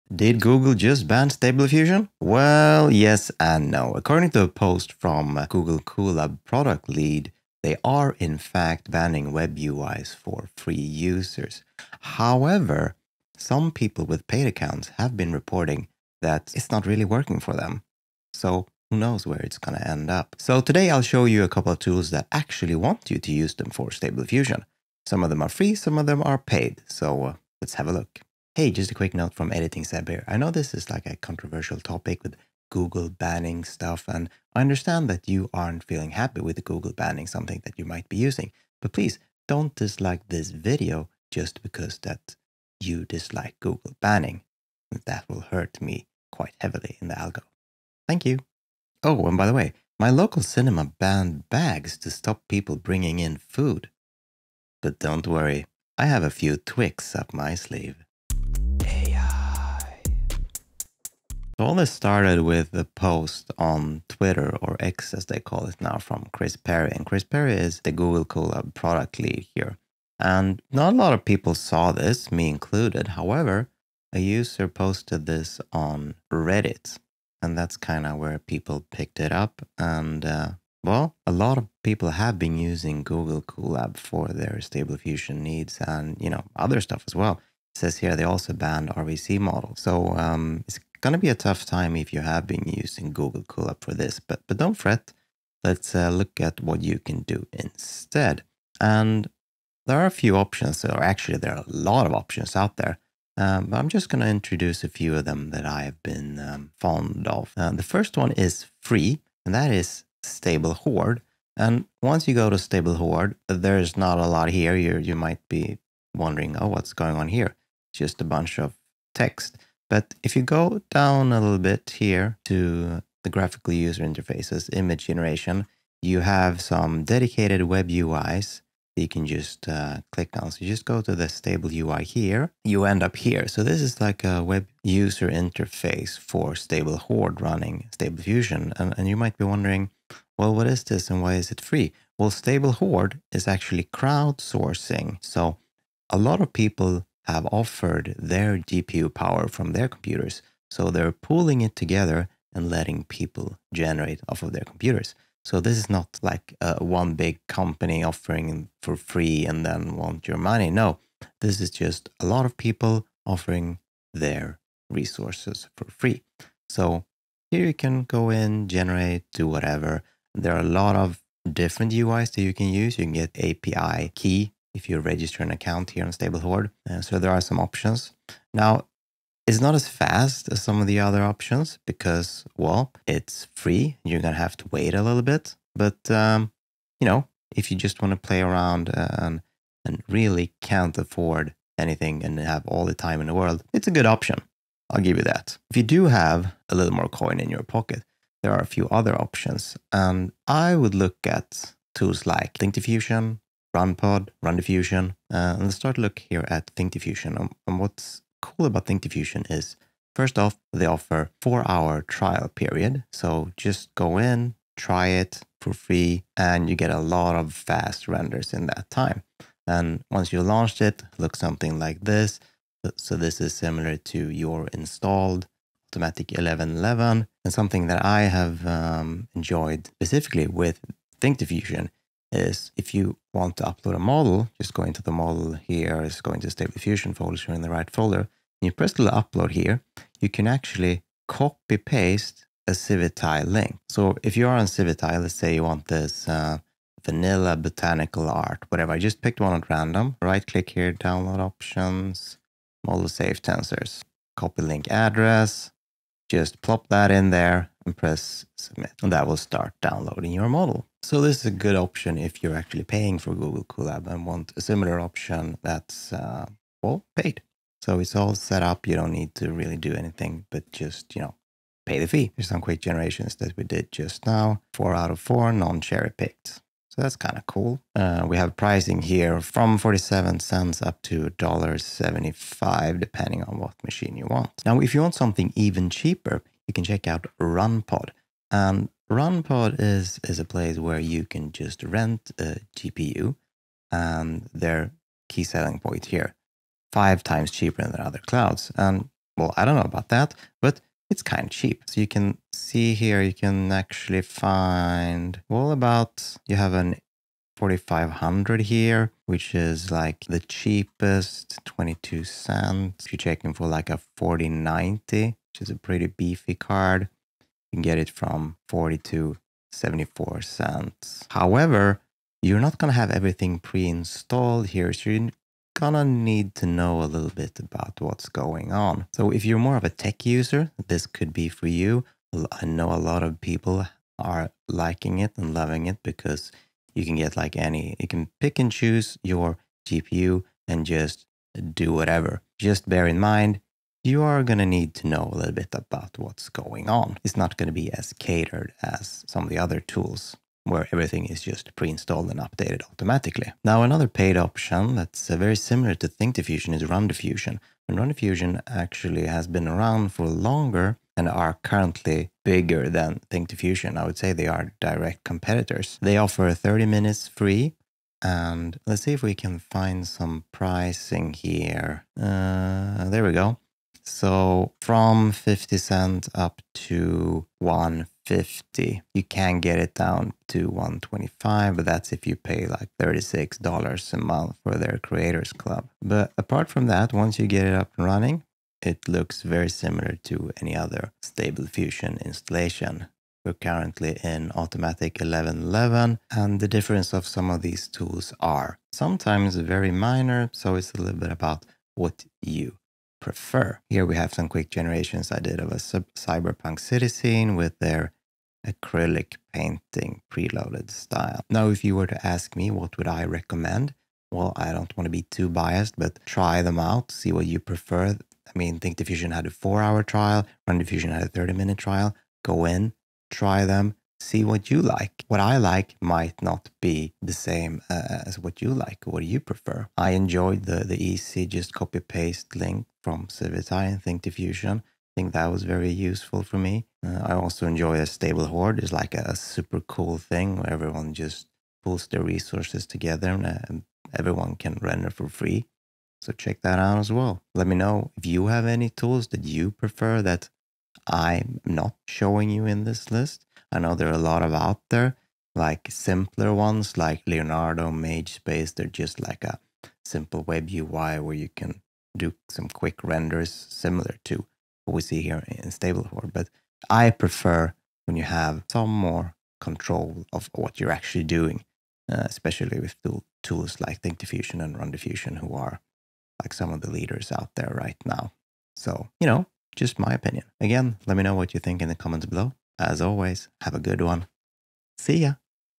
Did Google Just BAN Stable Diffusion? Top Online Stable Diffusion Tools.
Here are some options to use instead of Google Colab.

0:00 Google Ban & Dadjoke
1:47 Option 1 Stable Horde
3:25 Option 2 ThinkDiffusion
4:43 Option 3 RunPod
5:52 Option 4 RunDiffusion